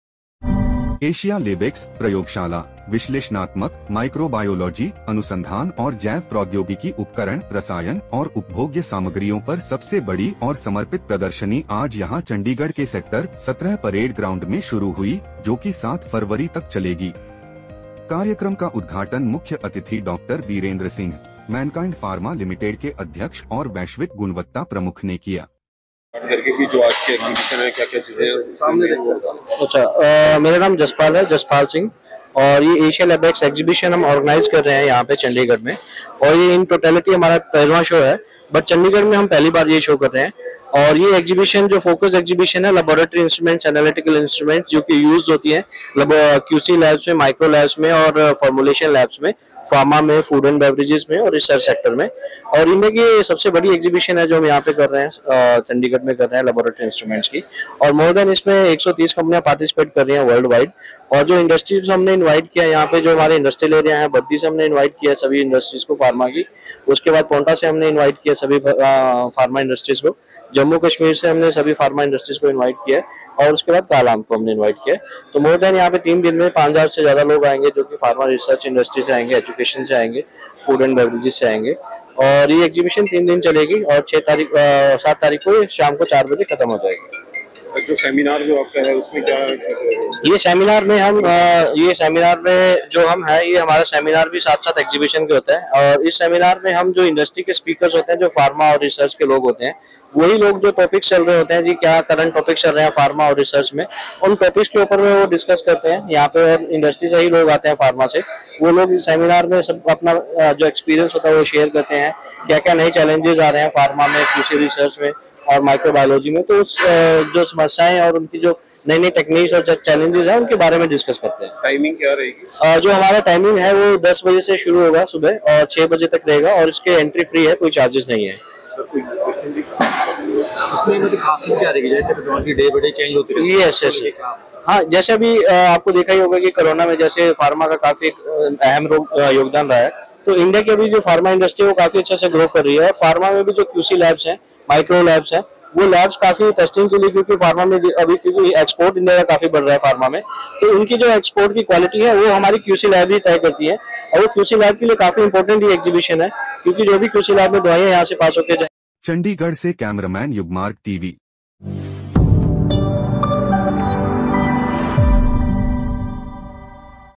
फ़ेंज़ा एग्जिबिशन प्रा. लिमिटेड का एशिया लेबेक्स 2024 मेगा लेबोरेटरी शो चंडीगढ़ में शुरू | YUGMARGTV
एशिया लेबेक्स, प्रयोगशाला, विश्लेषणात्मक, माइक्रोबायोलॉजी, अनुसंधान और जैव प्रौद्योगिकी उपकरण, रसायन और उपभोग्य सामग्रियों पर सबसे बड़ी और समर्पित प्रदर्शनी आज यहां  चंडीगढ़ के सेक्टर-17 परेड ग्राउंड में शुरु हुई, जोकि 7 फरवरी तक चलेगी। कार्यक्रम का उद्घाटन मुख्य अतिथि डॉ. बीरेंद्र सिंह, मैनकाइंड फार्मा लिमिटेड के अध्यक्ष और वैश्विक गुणवत्ता प्रमुख ने किया।

मेगा शो के बारे में जानकारी देते हुए  एशिया लेबेक्स के निदेशक जसपाल सिंह ने बताया कि सेमिनार का विषय 'विज्ञान उद्योग अनुसंधान और अनुप्रयोग के बीच अंतर को भरना' है। यह सेमिनार क्षेत्र में नए, उत्साहजनक और भविष्य के विकास को प्रदर्शित करेगा, जो अकादमिक, फार्मास्युटिकल उद्योग, अनुसंधान संस्थानों सीआरओ के बहु-विषयक शोधकर्ताओं के लिए एक अद्भुत अवसर होगा। उन्होंने आगे कहा कि नियामक संगठन अपनी सबसे अद्यतन अनुसंधान उपलब्धियों को प्रस्तुत करेंगे और क्यू.सी. और अनुसंधान एवं विकास प्रयोगशालाओं के नवीनतम रुझानों और नवाचारों पर विशेष जोर देने के साथ संबंधित क्षेत्र के विविध पहलुओं पर चर्चा और विचार-विमर्श करेंगे।